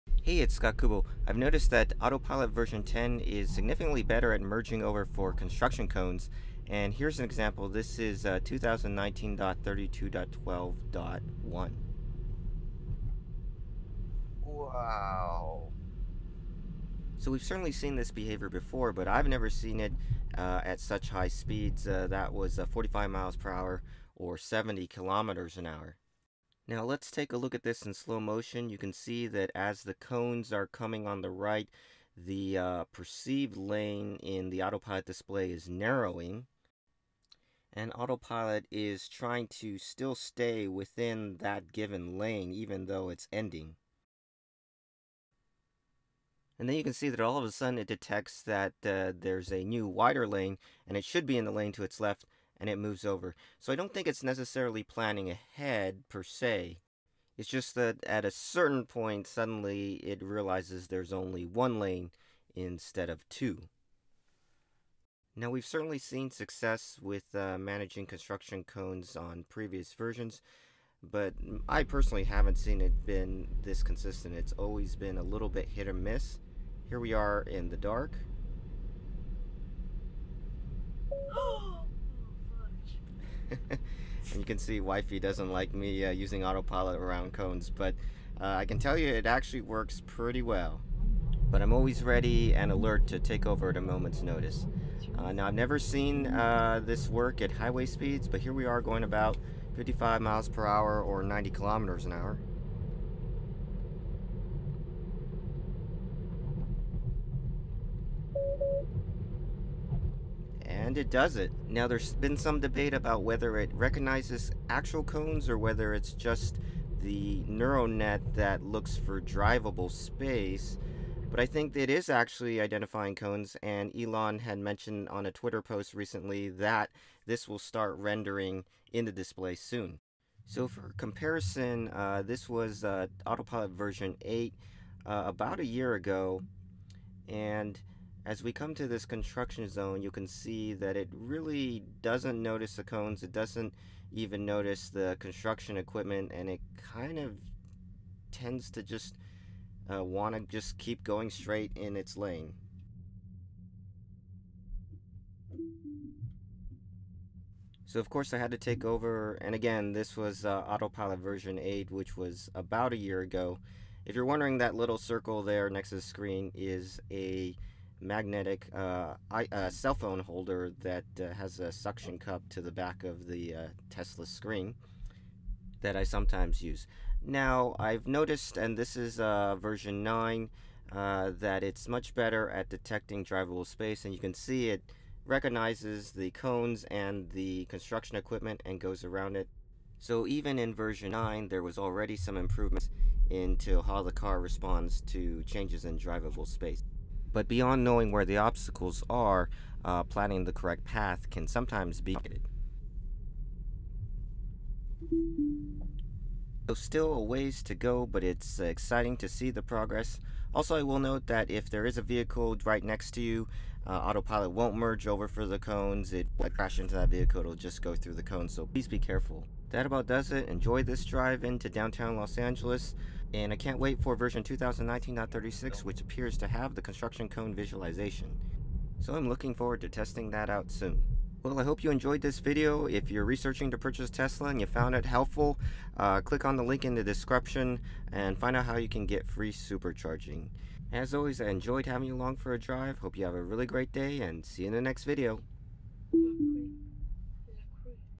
Tesla Autopilot V10 Navigating Construction Cones
Ordering a Tesla? Get 1000 miles or 1600 km of FREE supercharging

IPOW Magnetic Cell Phone Mount Holder With Soft Firm Goose Arm